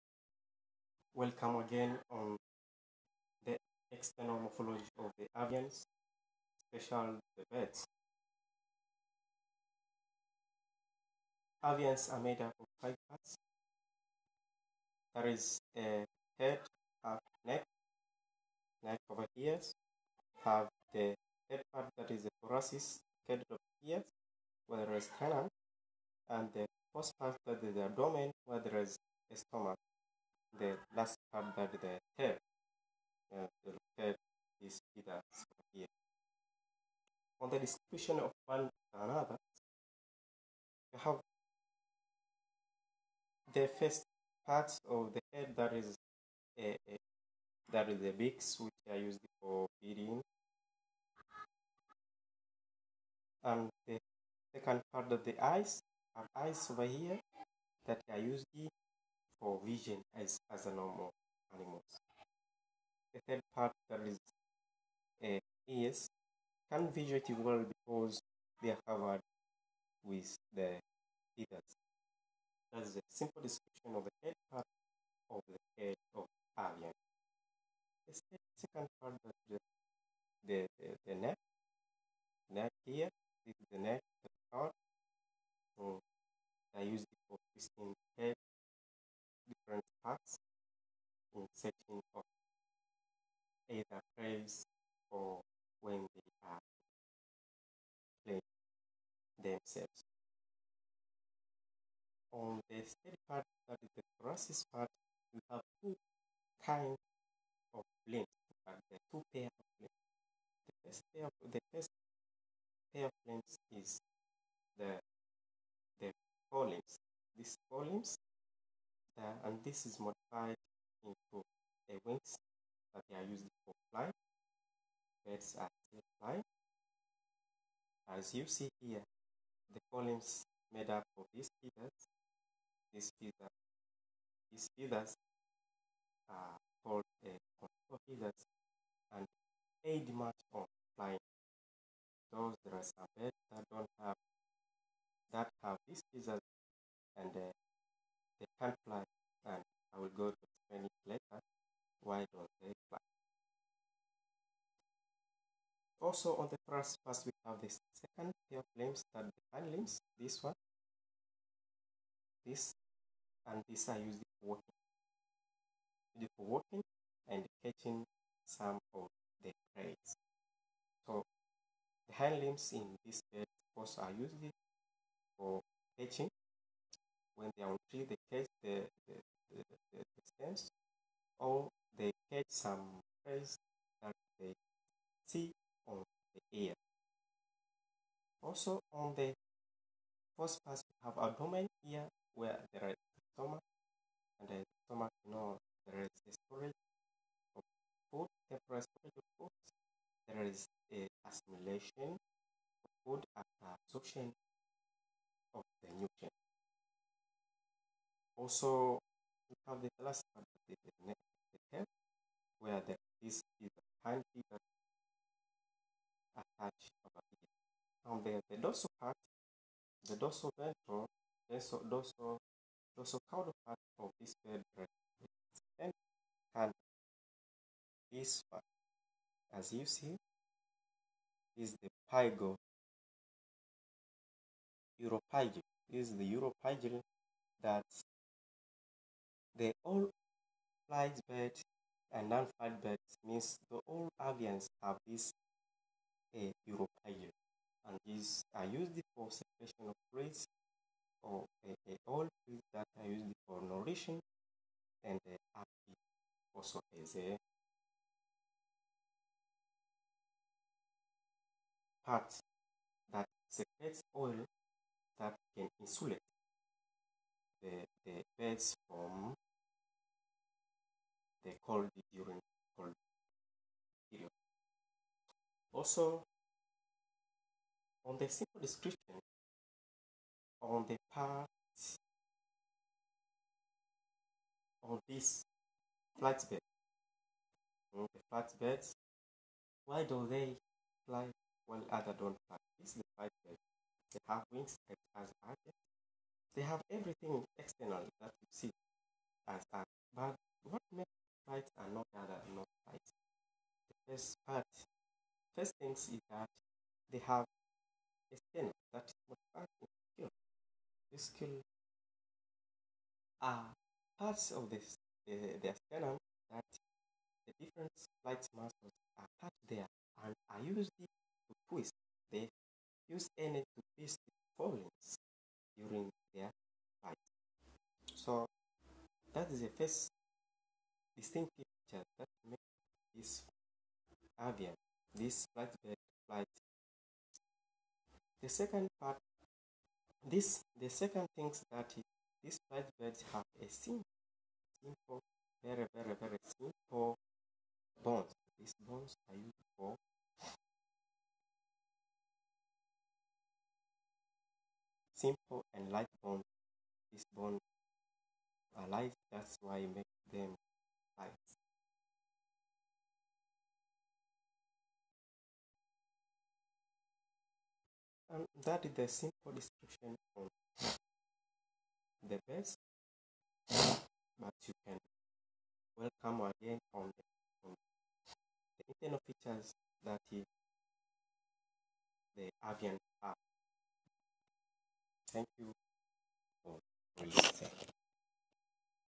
Avian Morphological Features
That's all about external appearence of phylum Aves and distinction between the fligt and Retites birds